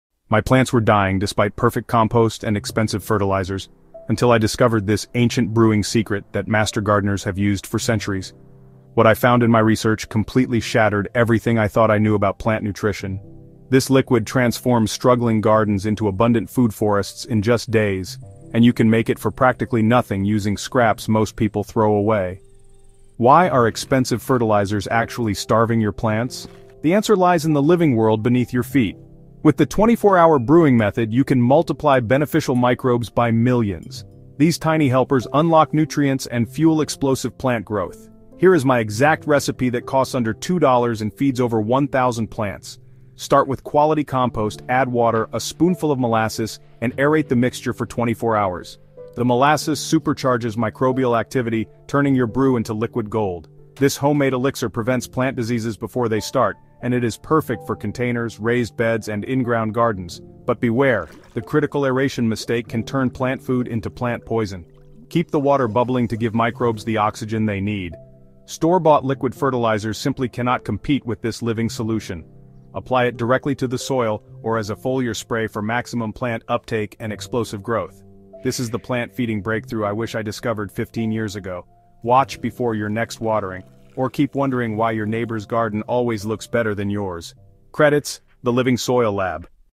Dead Soil? This Plant Will Bring It Back to LIFE 🌿
Your soil isn’t dead—it’s sleeping. Discover how comfrey and other “dynamic accumulators” can reawaken the biological life in your garden soil. It’s not magic—it’s biology.